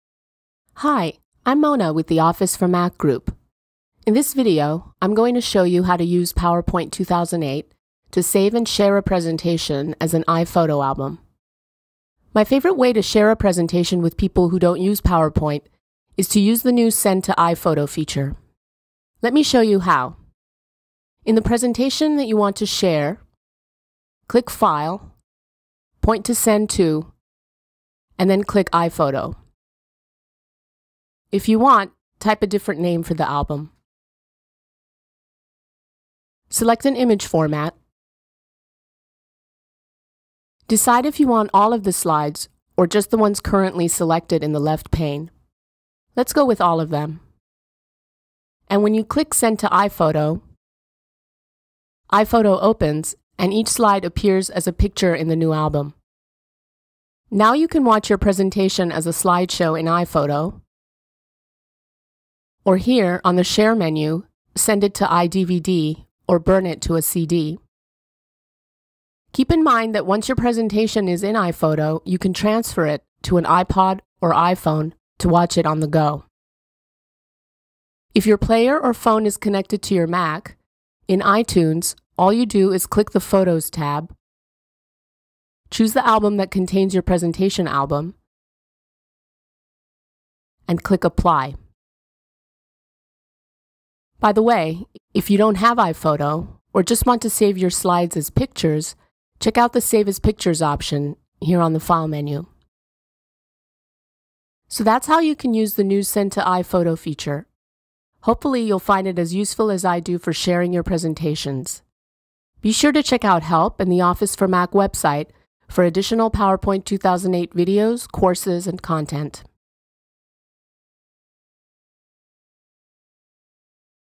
Share PowerPoint slides with iPhoto
Share PowerPoint presentations with iPhoto. View your presentations on the go with your iPhone.